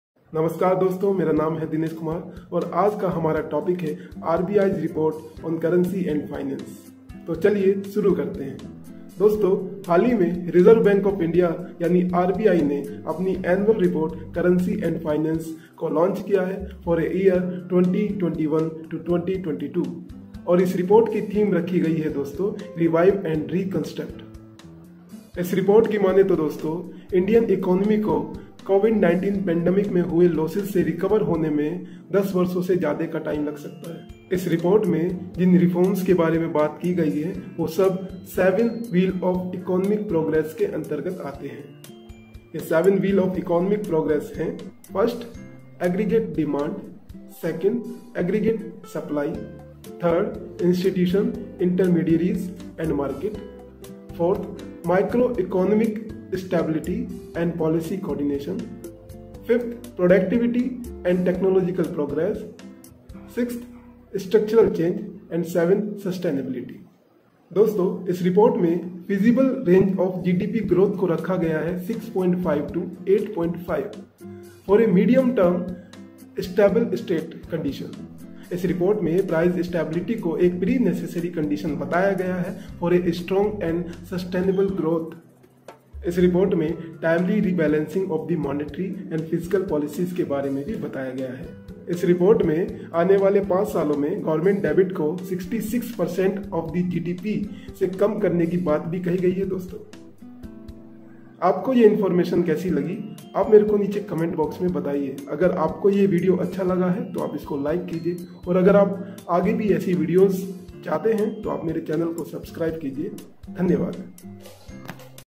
RBI's Report on Currency & Finance 2021-2022 || आरबीआई की मुद्रा और वित्त पर रिपोर्ट 2021-2022
RBI has released its Annual report on Currency & Finance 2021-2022
To get more about it, Watch the video😇😇😇

आरबीआई ने मुद्रा और वित्त पर अपनी वार्षिक रिपोर्ट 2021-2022 जारी की है
इसके बारे में अधिक जानने के लिए, वीडियो देखें:---

Happy Learning
ThankYou DoSTo 😇😇😇